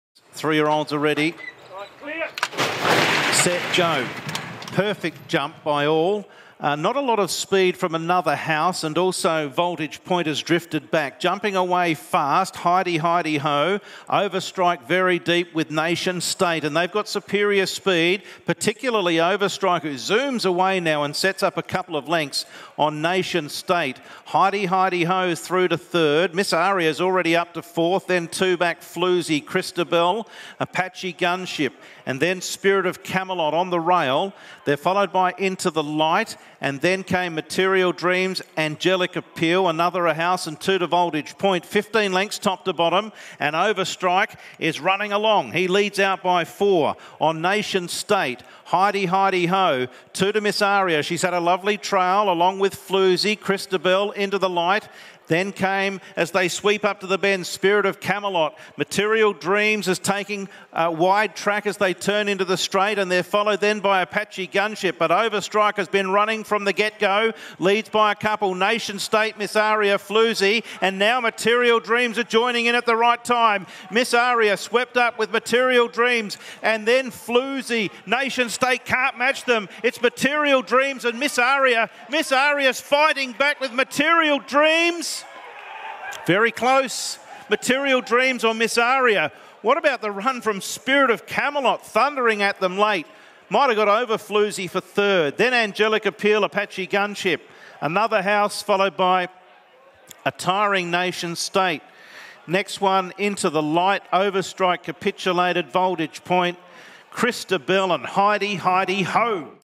HC Nitschke Stakes 2024 - MATERIAL DREAMS (3YO OPEN SWP LR) Listed Morphettville 20 April
$120,000 – H C Nitschke Stakes 2024 (3YO OPEN SWP LR) Listed Morphettville 1400m 20/04/2024 won by MATERIAL DREAMS (AUS) Ridden by Jacob Opperman and Trained by Nick Ryan

Results:
1st - Material Dreams (54kg) - $65,495
2nd - Miss Aria (54kg) - $21,250
3rd - Spirit Of Camelot (54kg) - $10,750
4th - Floozie (54kg) - $6,000

Horse – Material Dreams (AUS)
SIRE - Lean Mean Machine (AUS)
DAM - Lincoln's Sunset (NZ)
COLOUR – Brown or Black
FOALED - 15/10/2020
AGE - 3YO
SEX - Filly
CAREER – 10:5-0-0
PRIZE MONEY – $195,895
OWNERS – G J Lewis & Mrs J W Lewis